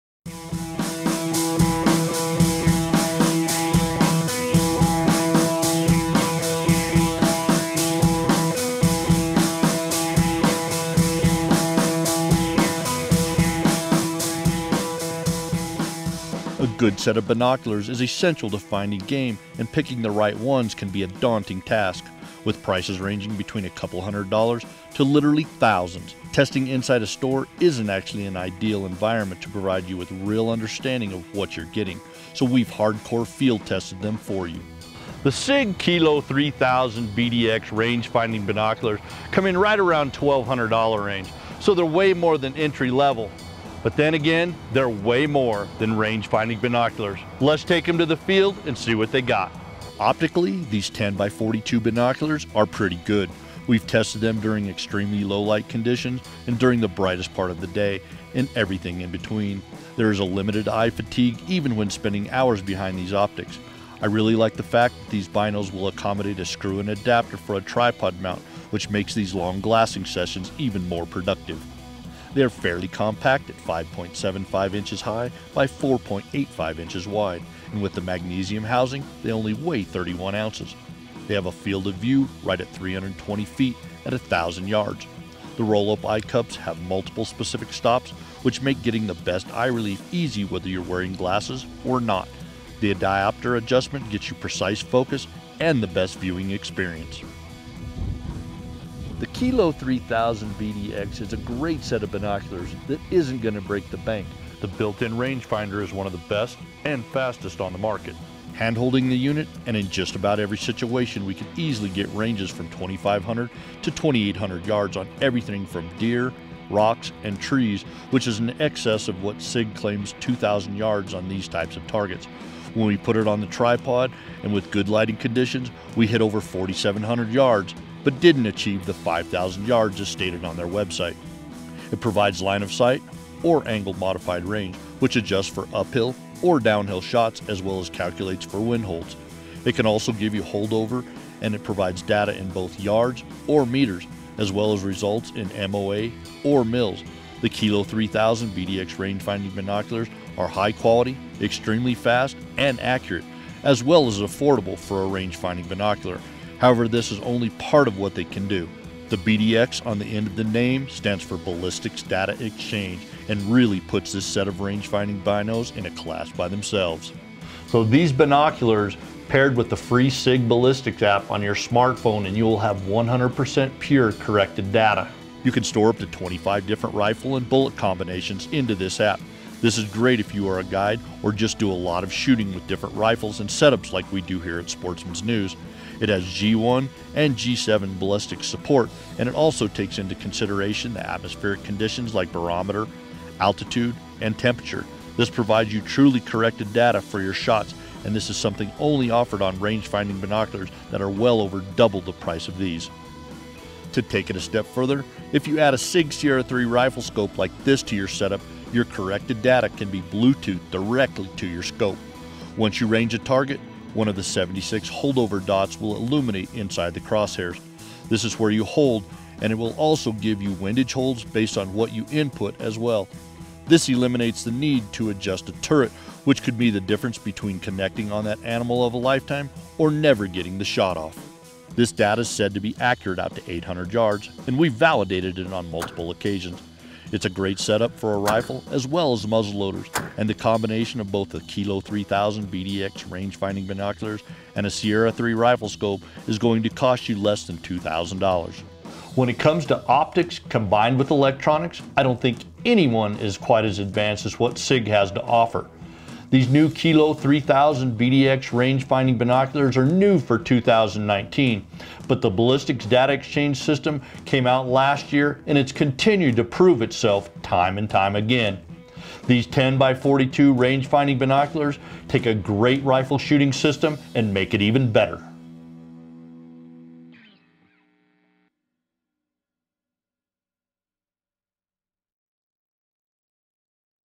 Sig Kilo3000BDX 10X42 Rangefinding Binoculars Review
The new Sig Kilo3000BDX Rangefinding Binoculars combine technologically advanced software with the excellent clarity you look for when buying a pair of binoculars.

Become a Sportsmans News PRO MEMBER today!